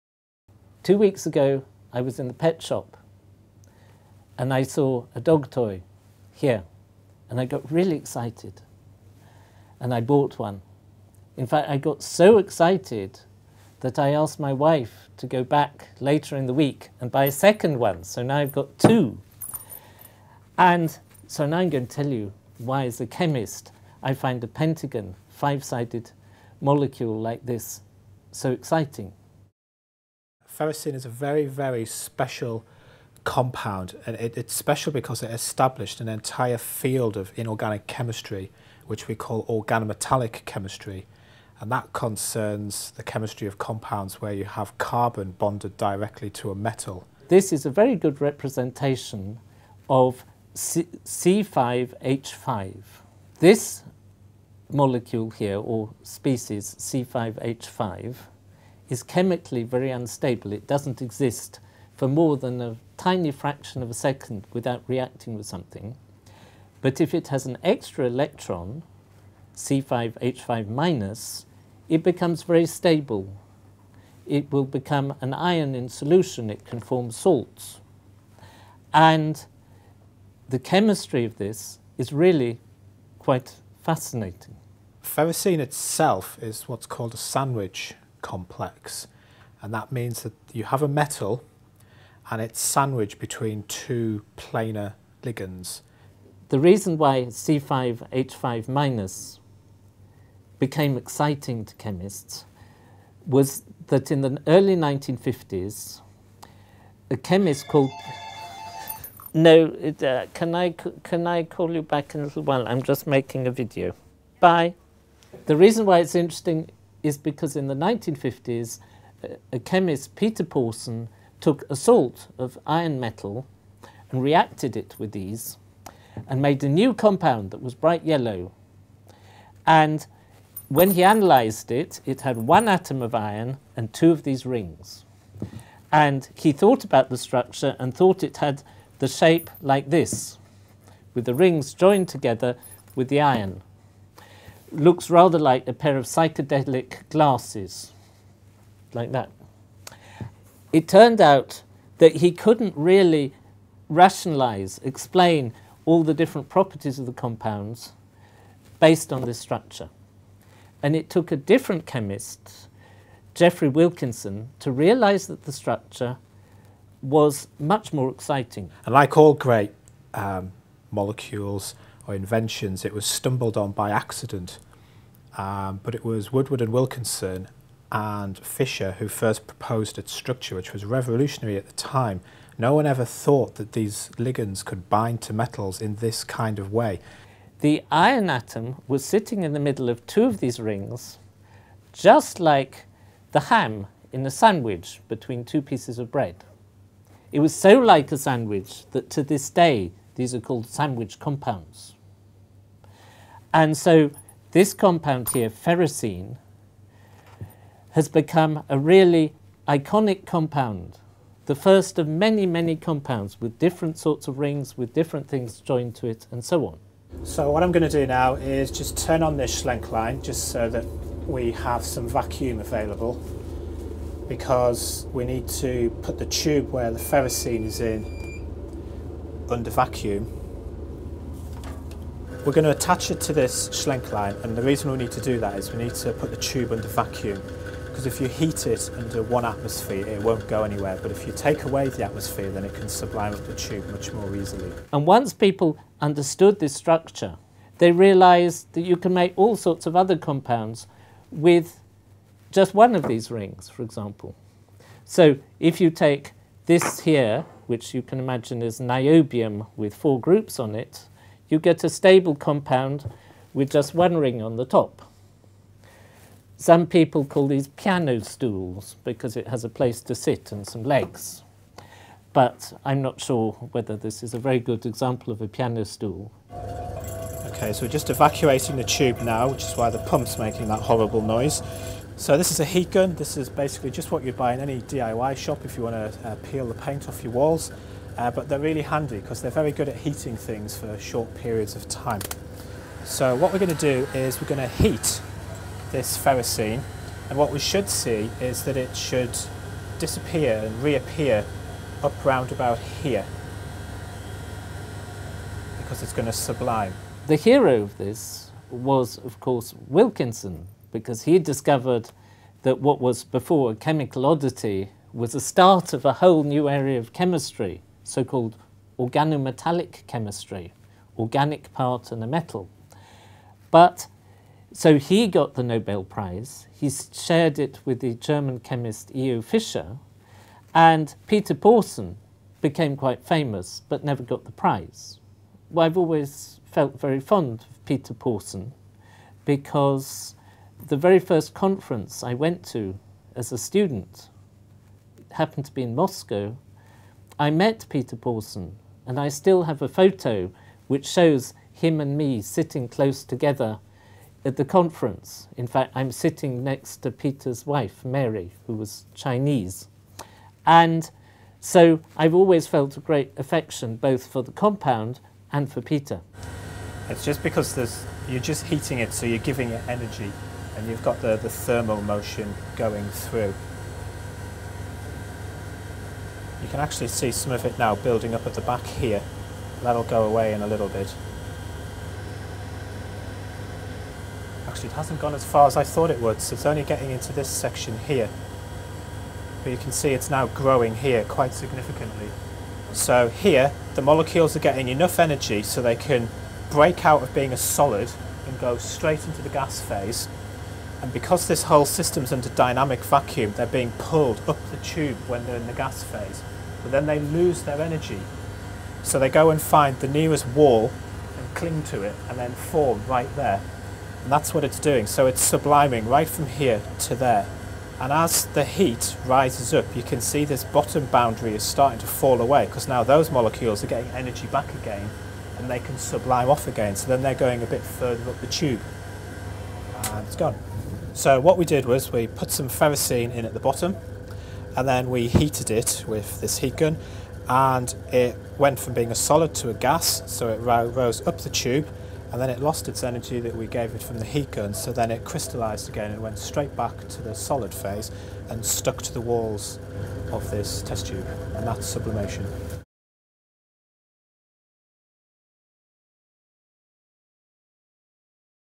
Ferrocene - Periodic Table of Videos
Discover how Ferrocene launched a whole new field of chemistry (and gave The Professor an excuse to buy more dog toys!).